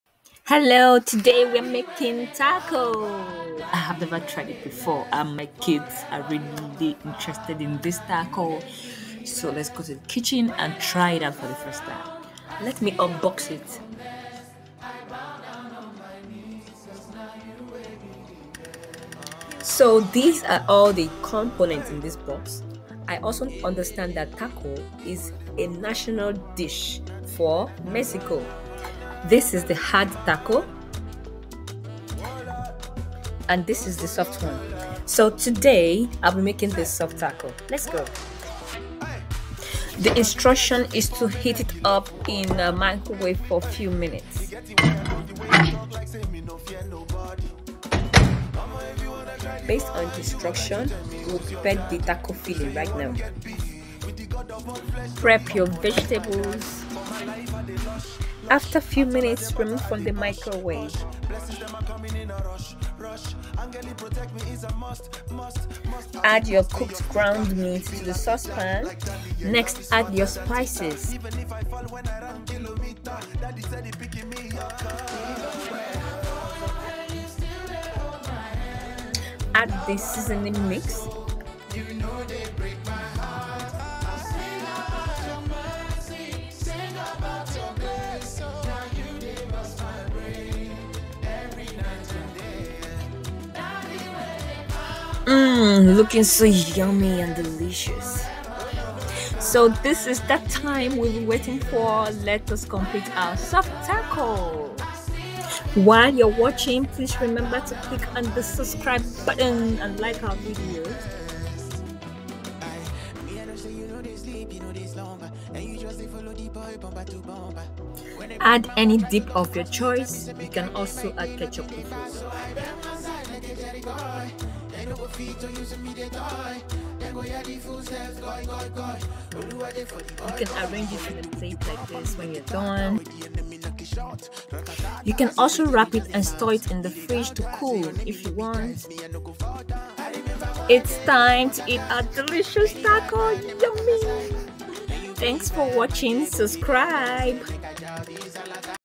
Can You Really Make the PERFECT Taco at Home?
Taco Tuesday is known as Mexican food. Watch how to make this soft yummy Tacos